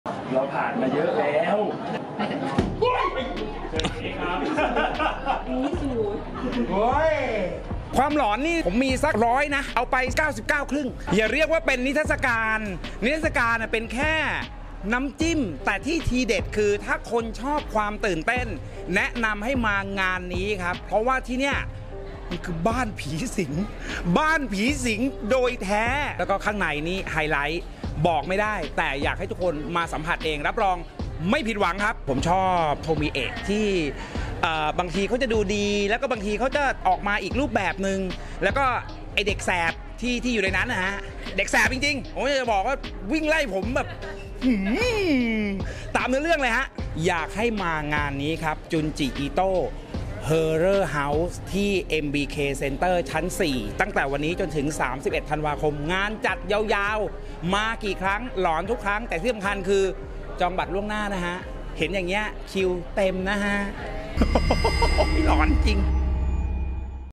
พี่แจ็ค และ ทีม The Ghost Radio “นิทรรศการคลังสยอง จุนจิ อิโต้” ครั้งแรกของประเทศไทย
พี่แจ็ค และ ทีม The Ghost Radio การันตีความหลอนสุดขั้วไปกับ “นิทรรศการคลังสยอง จุนจิ อิโต้” ครั้งแรกของประเทศไทย !!!
วันนี้ - 31 ธันวาคม 2566
ศูนย์การค้า เอ็มบีเค เซ็นเตอร์ ชั้น 4
ช่องทางการจำหน่าย : THAI TICKET MAJOR
*เงื่อนไขการออกบัตรเป็นไปตามที่ไทยทิตเก็ตเมเจอร์กำหนดไว้
Junji Ito Horror House in Thailand 2023
7 October - 31 December 2566
MBK Center 4 floor